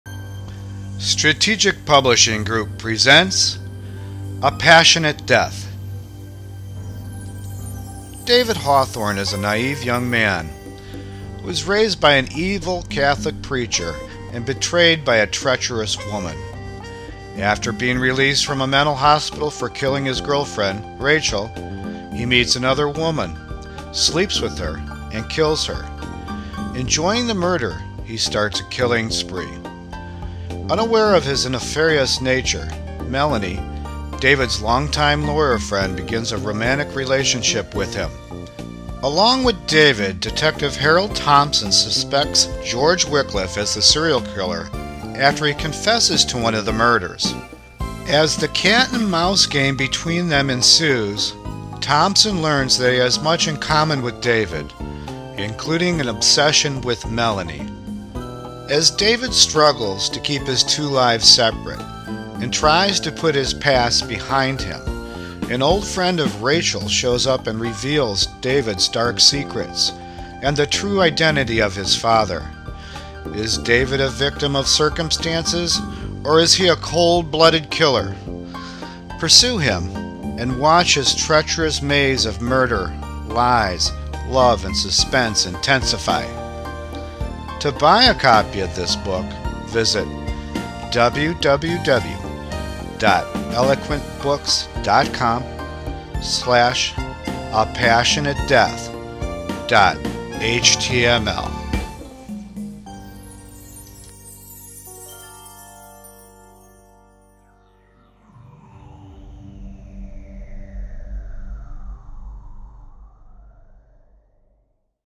A Passionate Death By Deepak Gupta
David Hawthorne is a naive young man who was raised by an evil Catholic preacher and betrayed by a treacherous woman. After being released from a mental hospital for killing his girlfriend, Rachel, he meets another woman, sleeps with her, and kills her.